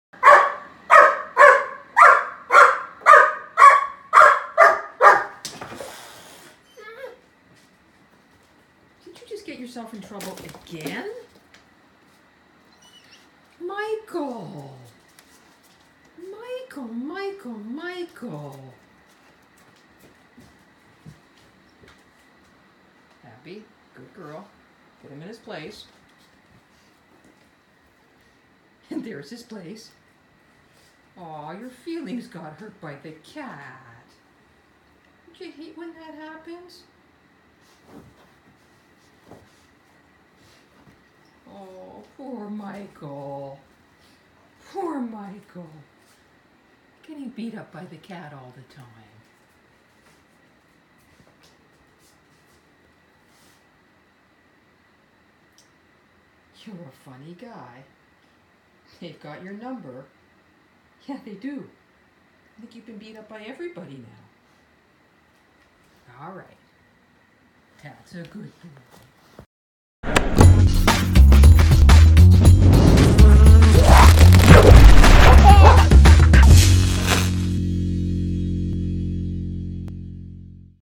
Typical cat hurts Great Dane puppy's feelings
Poor Mikey keeps getting in trouble with the cats. Watch and laugh as Abbie gives him a hiss and a swat for getting too close and personal. Mikey's feelings get hurt and he pouts on the sofa where he chases off Abbie.